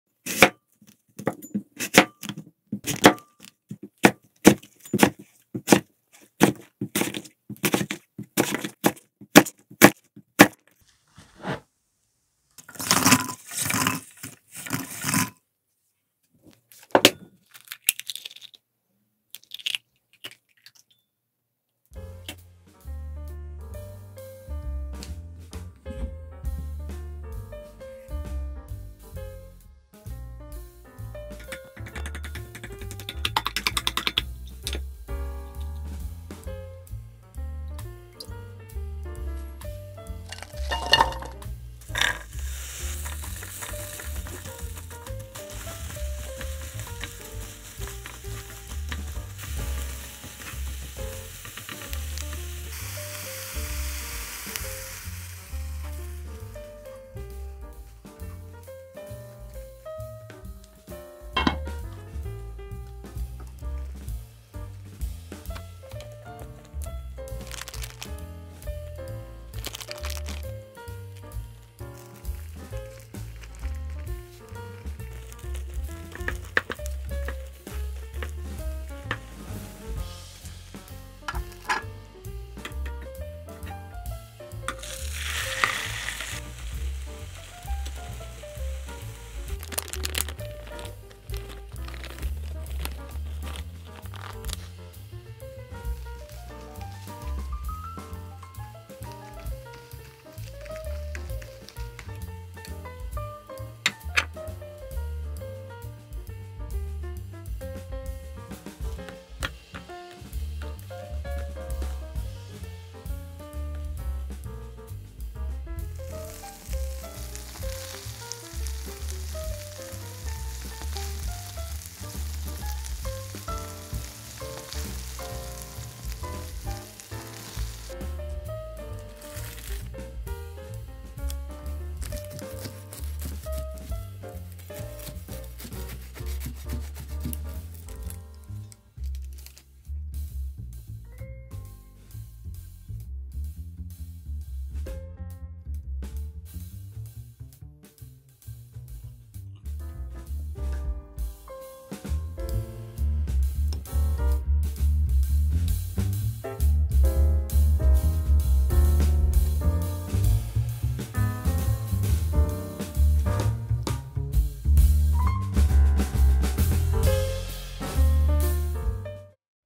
오래된 사과가 있으면 당장 꺼내세요! 사과 한 상자가 순식간에 사라집니다😋간단하고 영양만점 사과 계란 요리
⚠️ 레시피 활용시 출처를 남겨주세요 ☺️

🍅재료
사과 1개
계란 3개
토르티야 2장
모짜렐라 치즈
시나몬 가루
소금
아보카도오일

1 apple
3 eggs
2 tortillas
mozzarella cheese
Cinnamon Powder
salt
avocado oil

⭐⭐ 함께 보면 좋은 다이어트 레시피 영상들 ⭐⭐